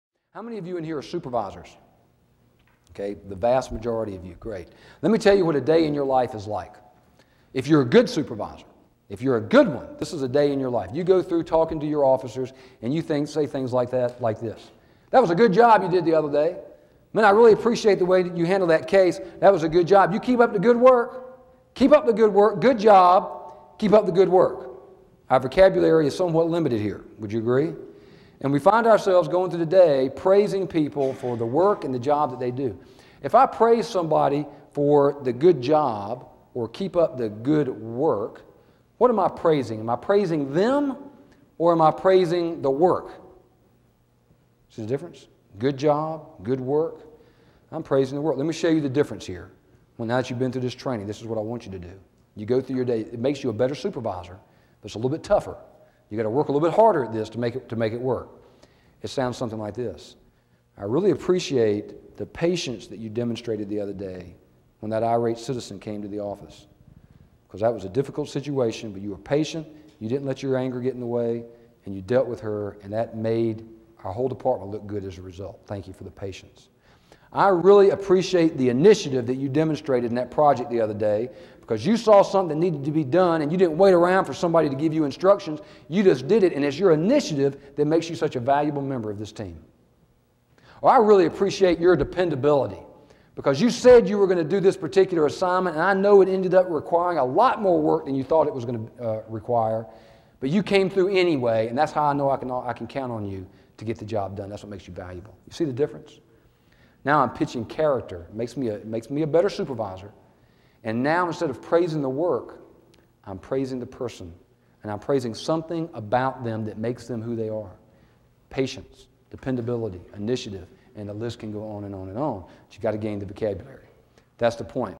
Character-based Supervision
Learning the vocabulary of character and how to praise for character instead of achievement makes you a better supervisor. Watch this police training video as I give examples of character-based praise that will motivate your law enforcement officers and increase your supervision skills.

For more police training videos on character and ethics, go

Sheriff Ray Nash
Police Dynamics Institute